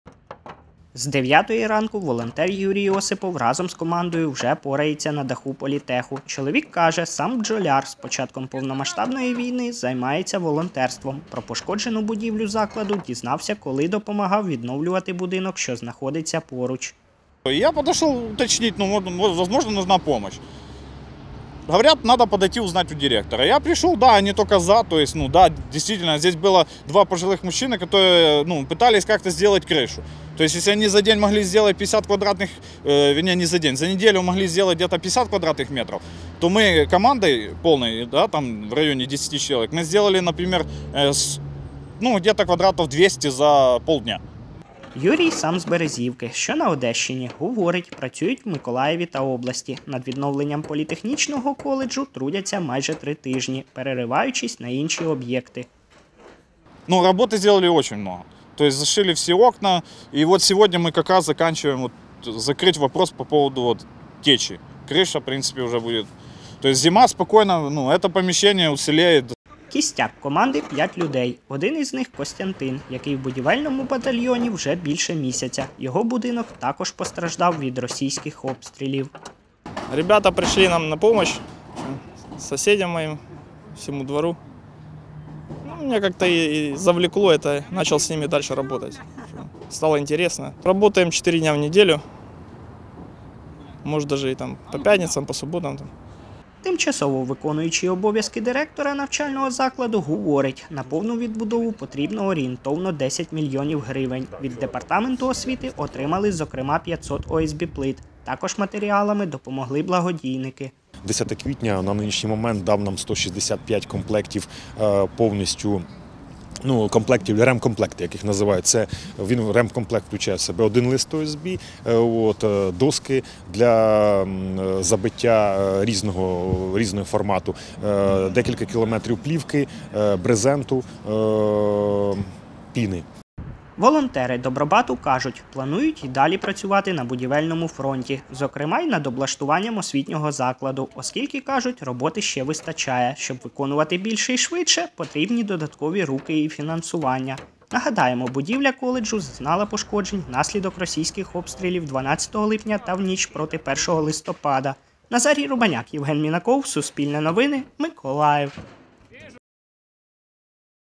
У Миколаєві волонтери ремонтують дах політехнічного коледжу, пошкодженого обстрілами
У Миколаєві волонтери команди "Добробат" перекривають пошкоджений внаслідок російських обстрілів дах Миколаївського фахового політехнічного коледжу. Вже облаштували понад 1000 квадратних метрів шифером. Працюють з вересня по декілька годин на день на волонтерських засадах.